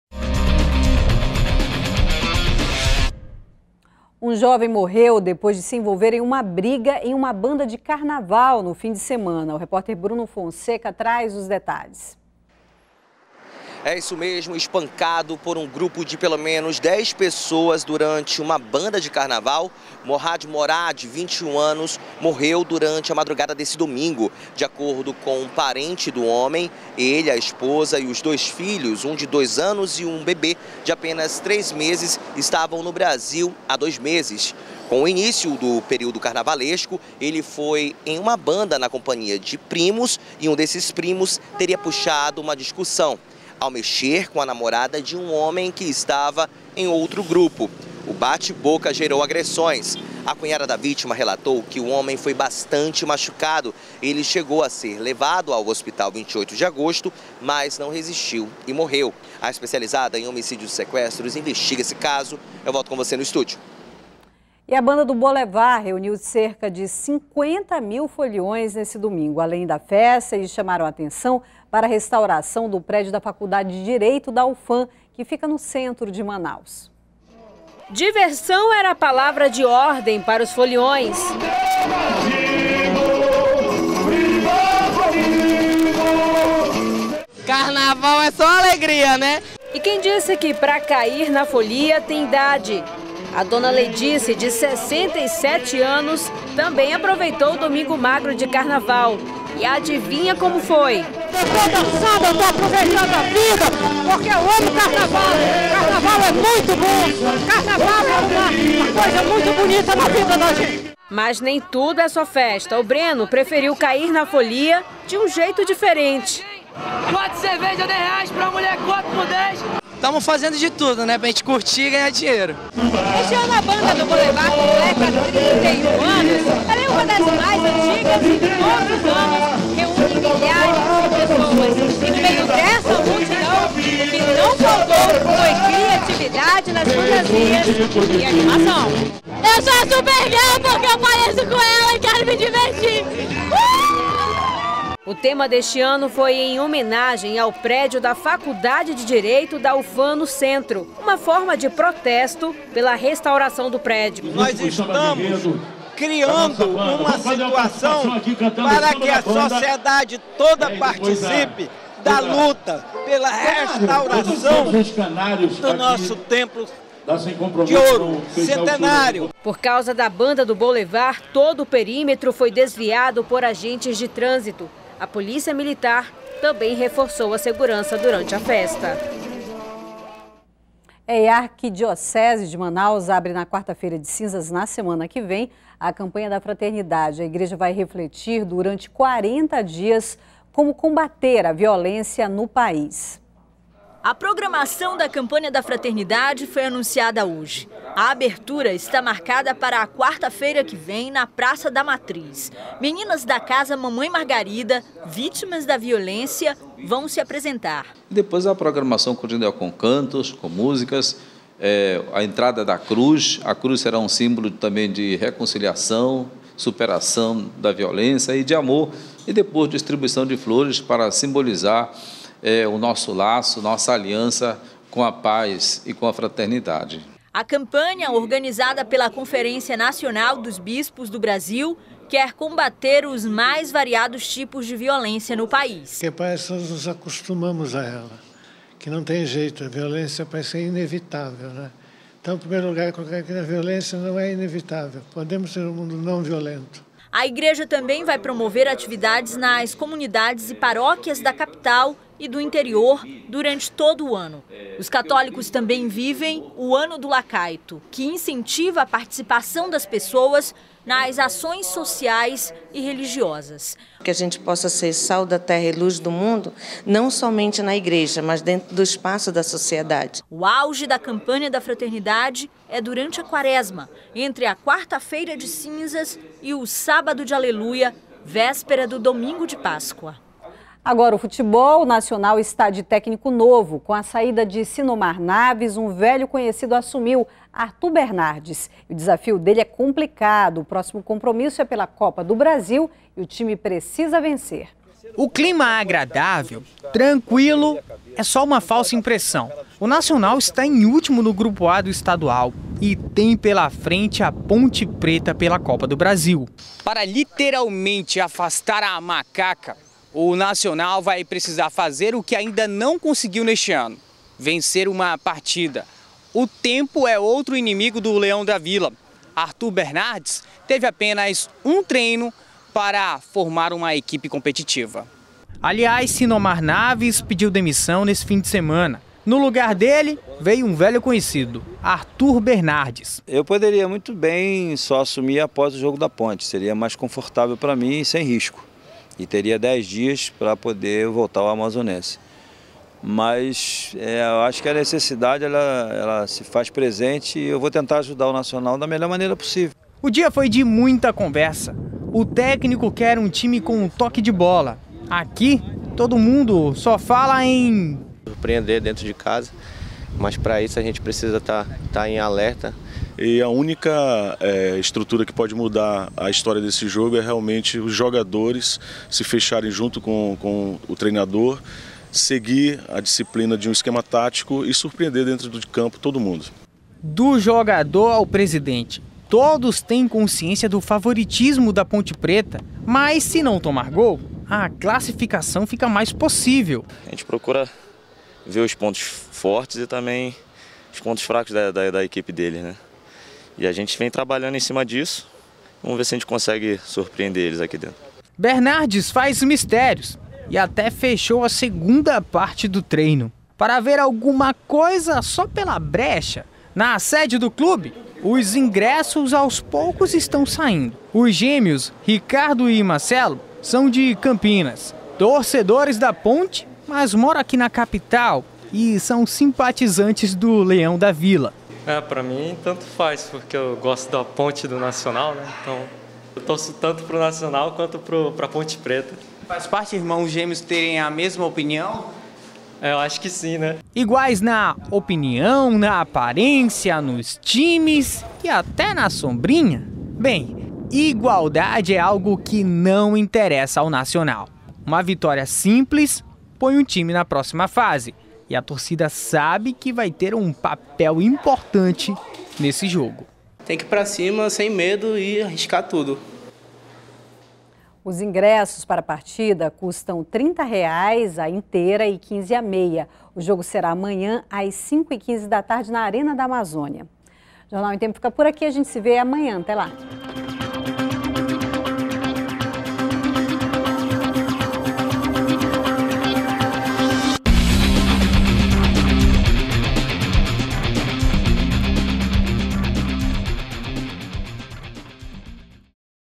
Novo técnico do Nacional assume com desafio de vencer partida pela Copa do Brasil em casa
03º BLOCO JORNAL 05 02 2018 - Novo técnico do Nacional assume com desafio de vencer partida pela Copa do Brasil em casa